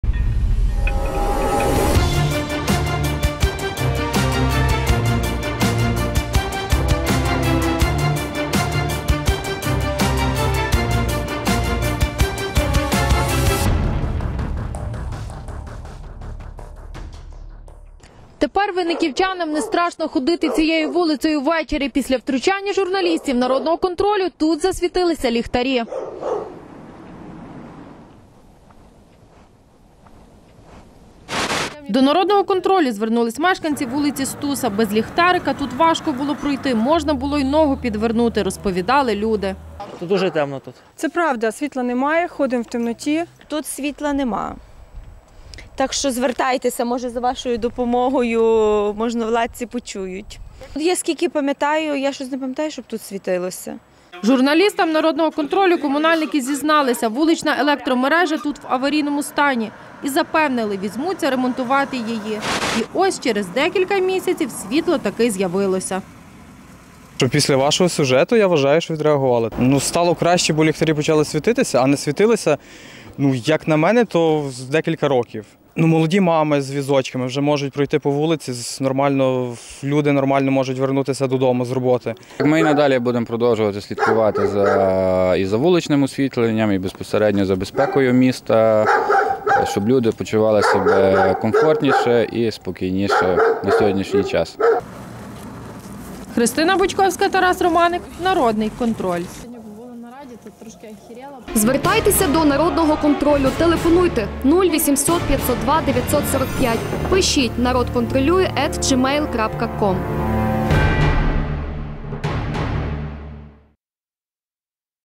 Після втручання Володимира Почаєвця та "Народного Контролю" на вулиці у Винниках запрацювали ліхтарі
По вулиці Стуса, що у Винниках довго не було освітлення. Мешканці звернулися до "Народного Контролю", аби місцева влада нарешті вирішила проблему. Через декілька місяців аварійну електромережу полагодили і тепер вуличні ліхтарі працюють.